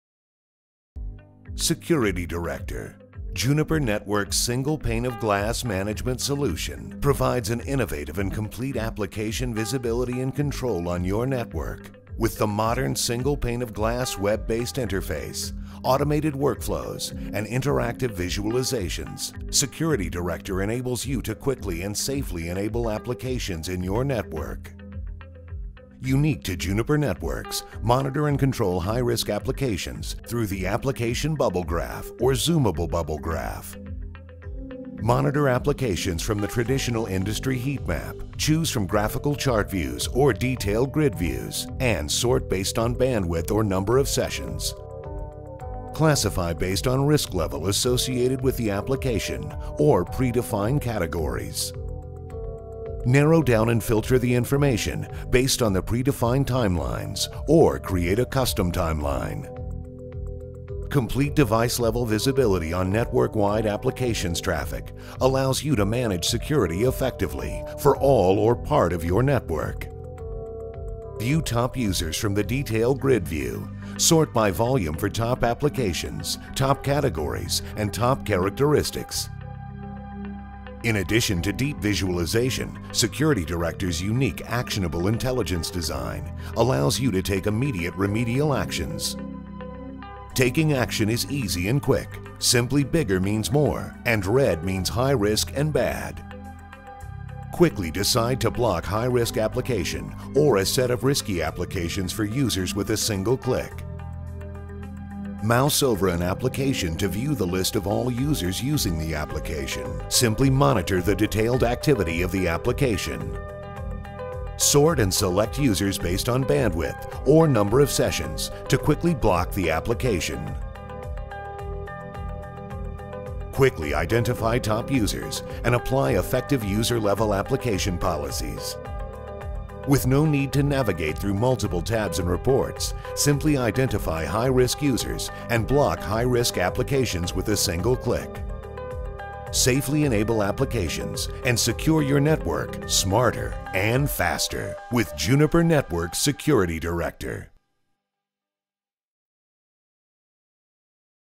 Security Director v16.1 Key Features Overview
Highlights of the key features and enhancements in the latest Juniper Networks Security Director release (v16.1).